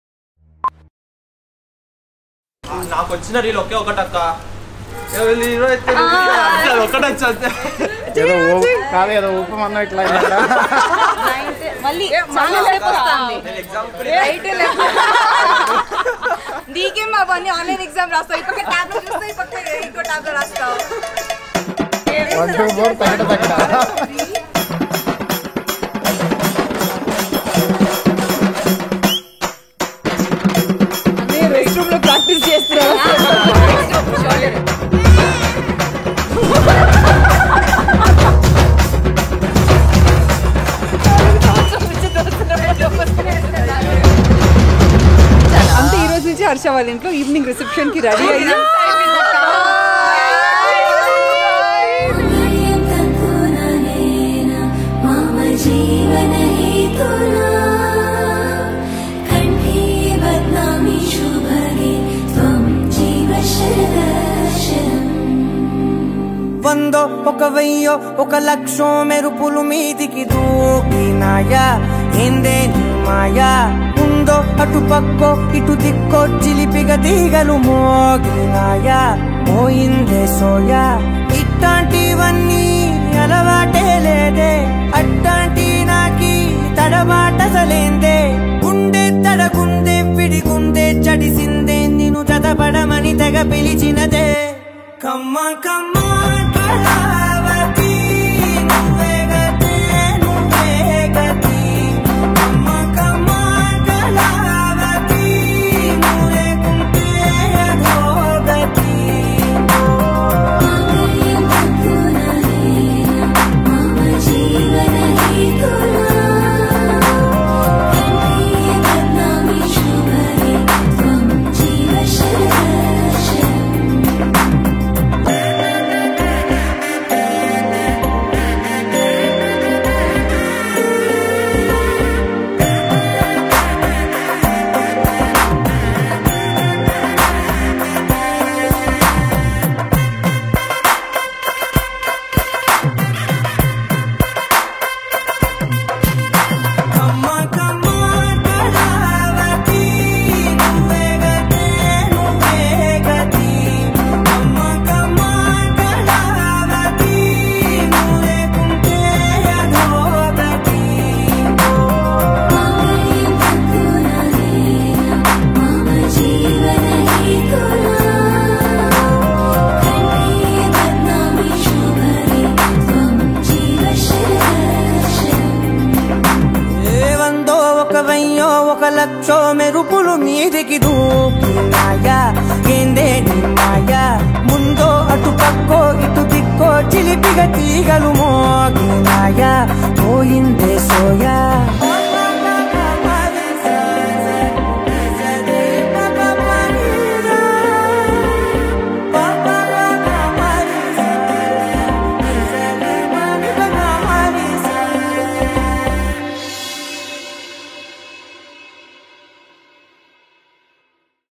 Haritha & Harsha | Wedding Film | Telugu Wedding | Zero gravity Photography
We are in awe of Haritha & Harsha Varma 's chemistry. They made this wedding shoot even more beautiful with their love and chemistry.

Watch this space as they serve us some couple-goals!